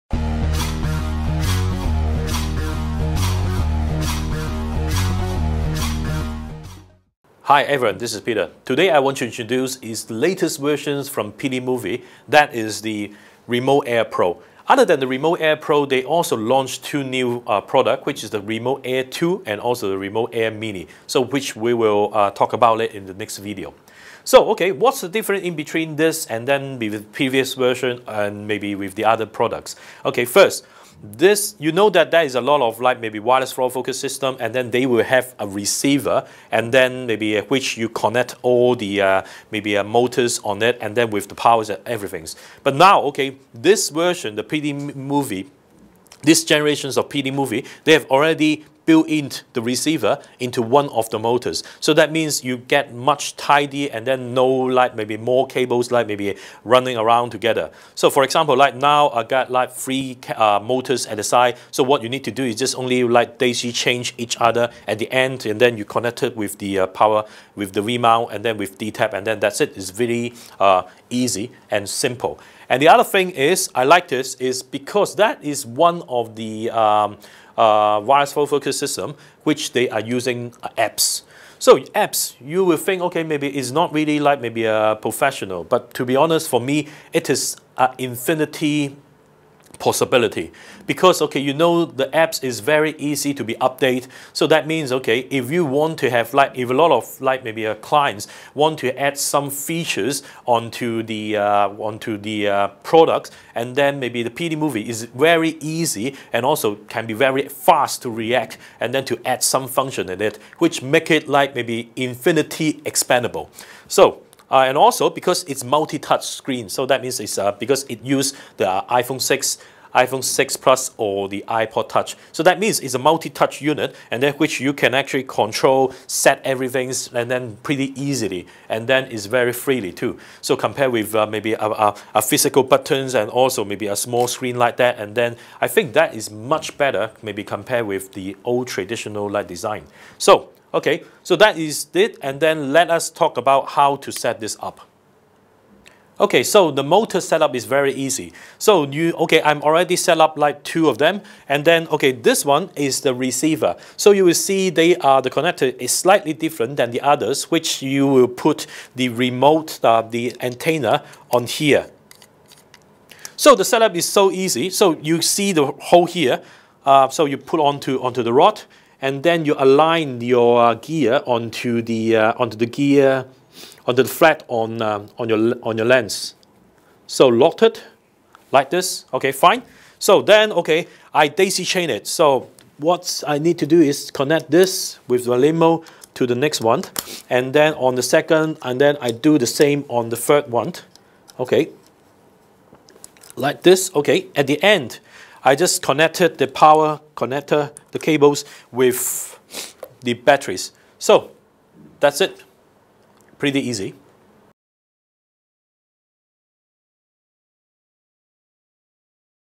Remote Air Pro Review Part 1
Remote Air Pro an innovative wireless follow focus.It offers two kinds of motor.  Only one motor/receiver is required for single channel use and the standard motors can be added for dual and triple channel use.